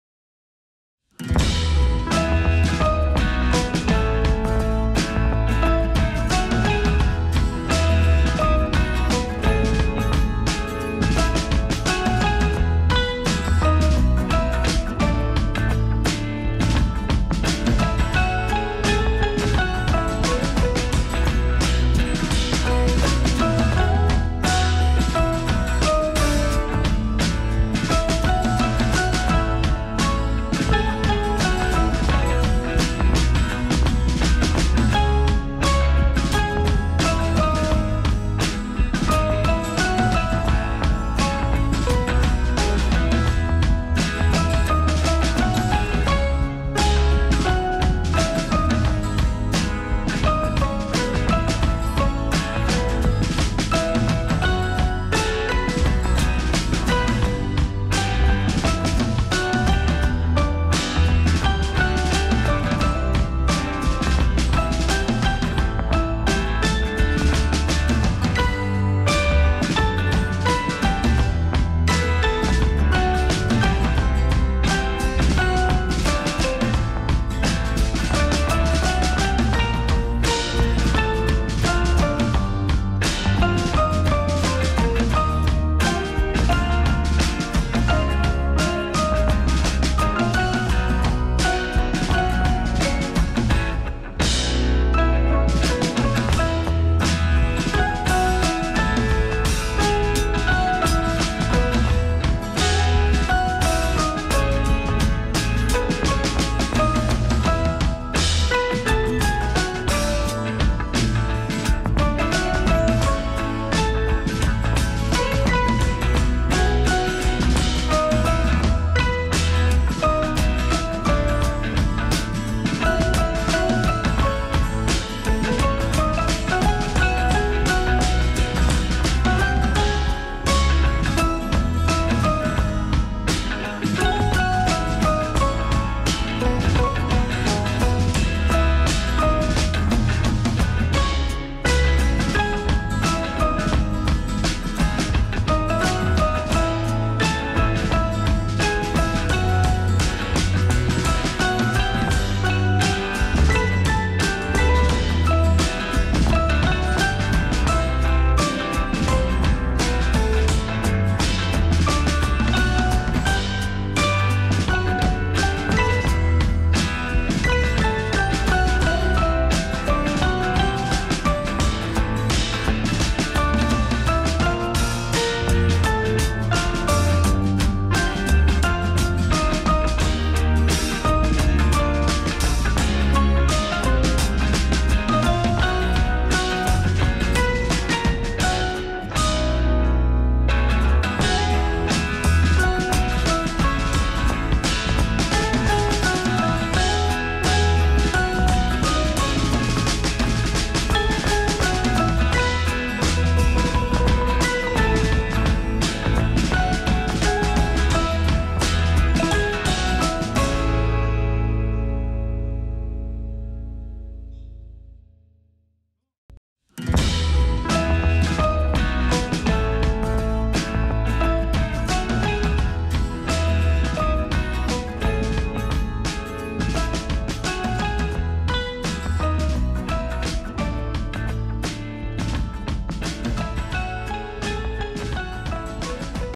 Cutest Red Panda Compilation with Fun Facts
A bushy tailed animal native to the eastern Himalayas and southwestern China, the red panda is also known as the lesser panda, red bear-cat, or red cat-bear.

Deforestation (the action of destroying large amounts of trees) has put Red Pandas in jeopardy. People who want to develop agriculture and those who down trees for loggers are destroying their natural environment.